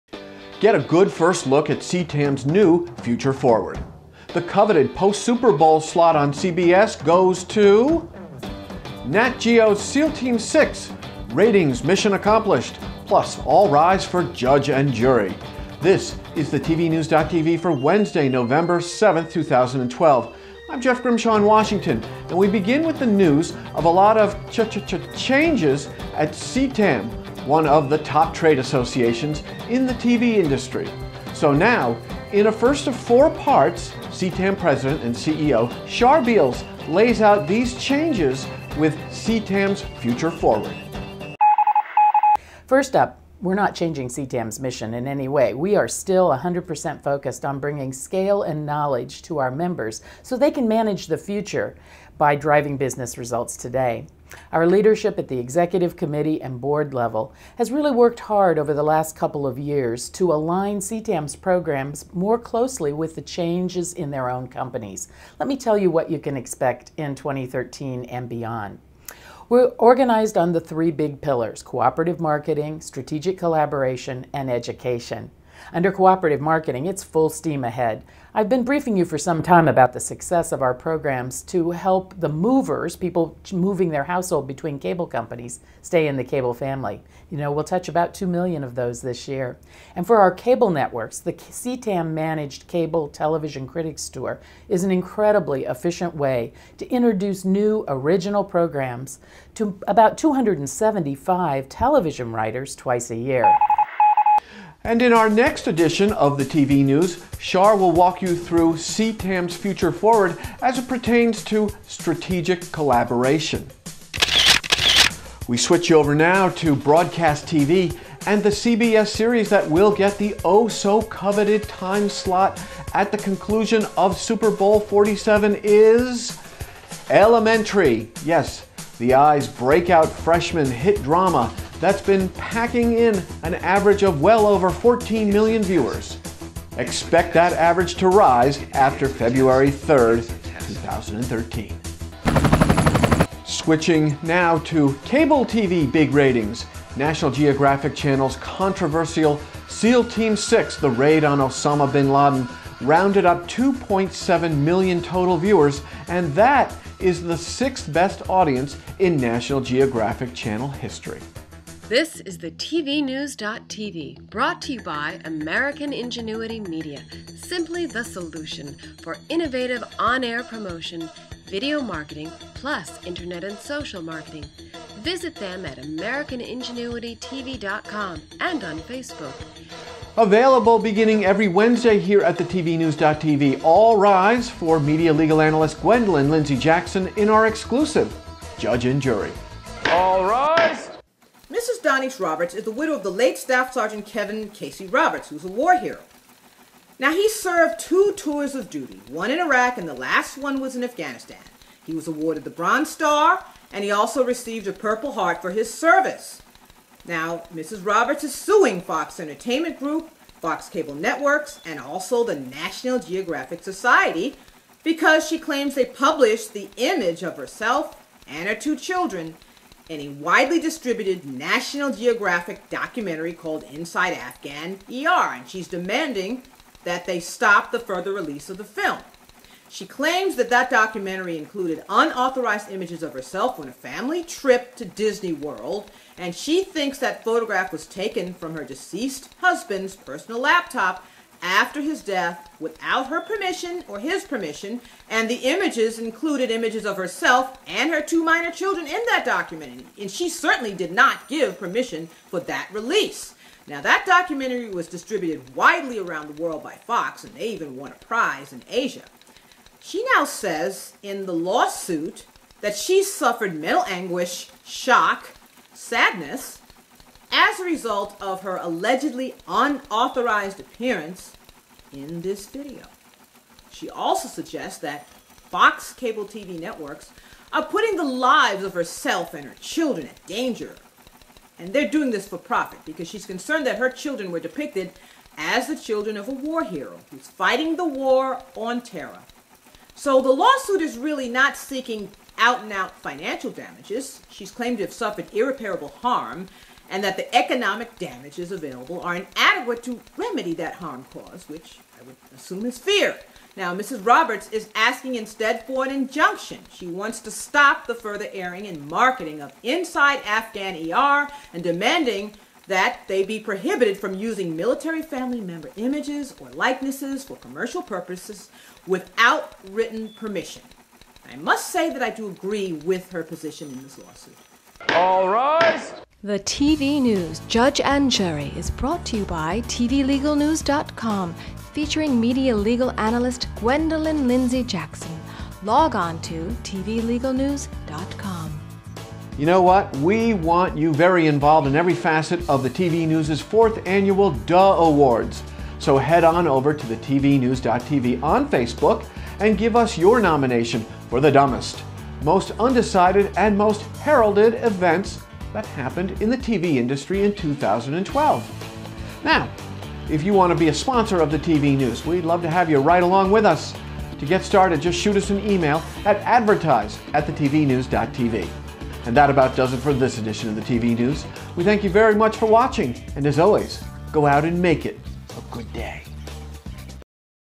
CTAM: Future Forward! Coveted Post Super Bowl XLVII Slot. TV News 11.7.12
In Part 1 of a Special 4 Part Series, CTAM President & CEO Char Beales outlines the new direction of CTAM - one of the top trade groups in the TV industry.  Which CBS series got the coveted Post-Super Bowl 47 time slot?   National Geographic Channel's "Seal Team Six": Ratings Accomplished!   Plus see why the wife of a US soldier, killed in action is suing FOX/National Geographic Channel.